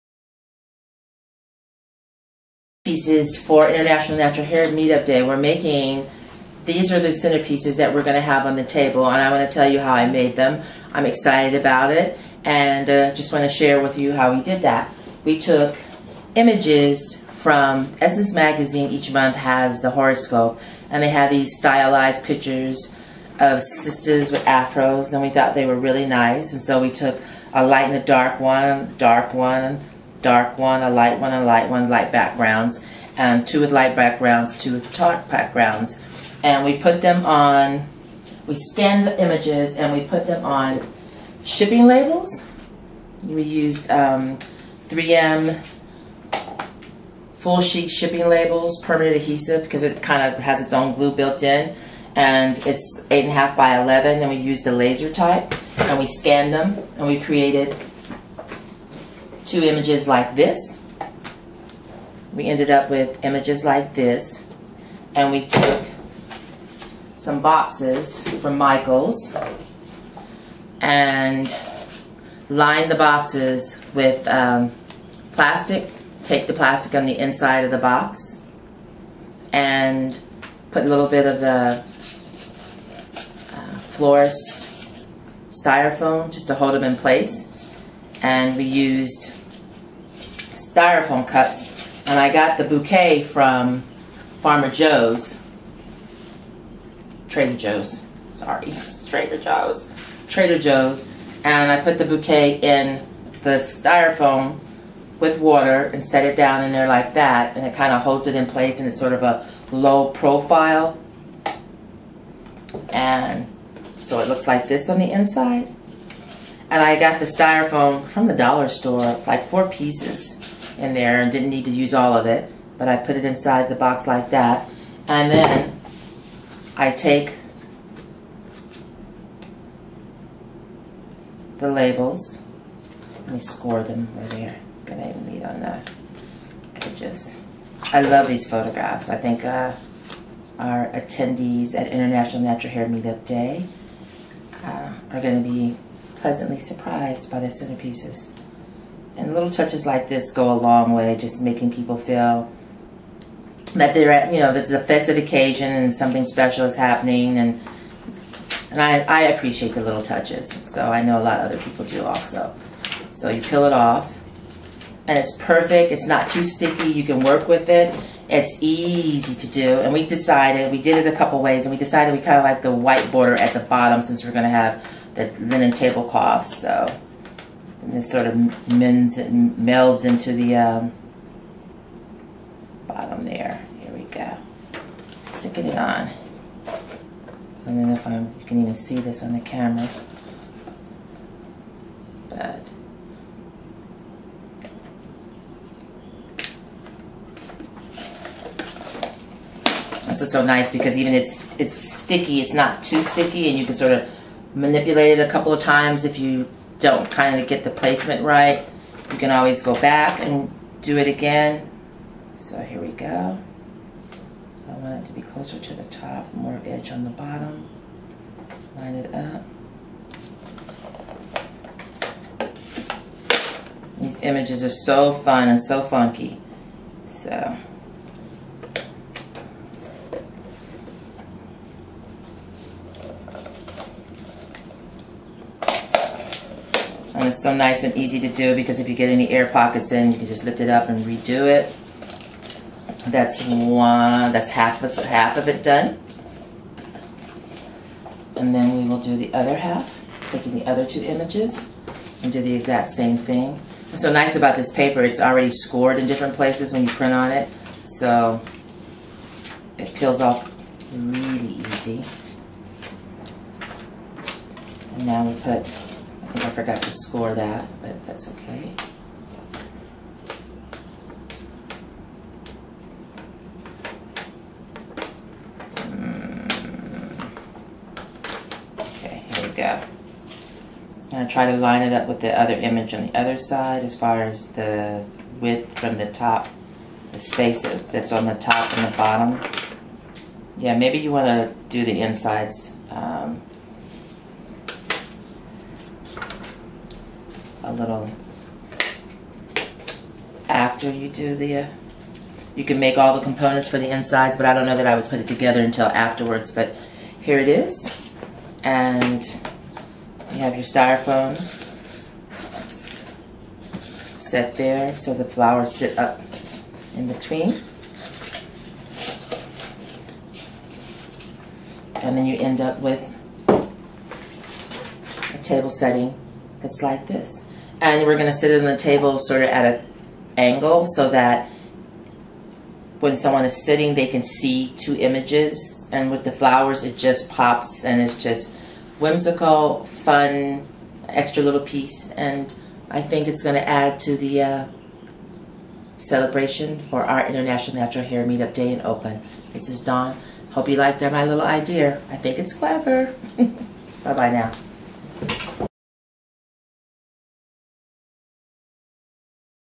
Stylish Center Pieces Easy To Create
A quick & easy center piece you can create for any occasion.